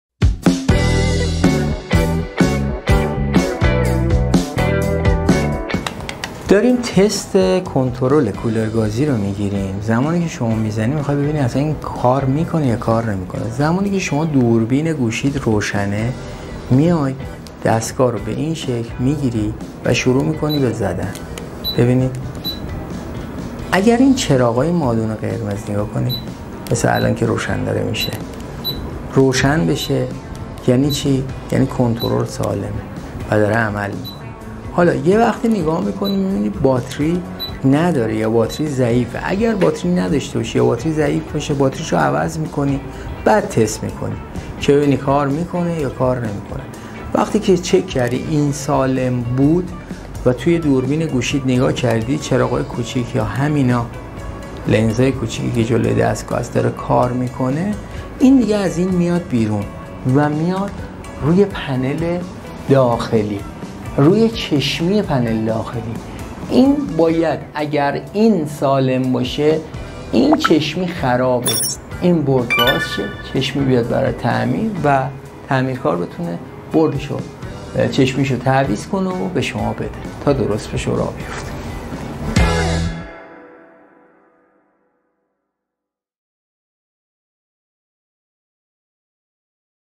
علت کار نکردن کنترل کولر گازی | آچاره
در برخی موارد ممکن است پس از استفاده از کنترل کولر گازی خود با کار نکردن آن مواجه شوید.در این ویدیو راهکار عیب یابی کنترل کولر گازی را بررسی میکنیم
شماره تماس سراسری 1471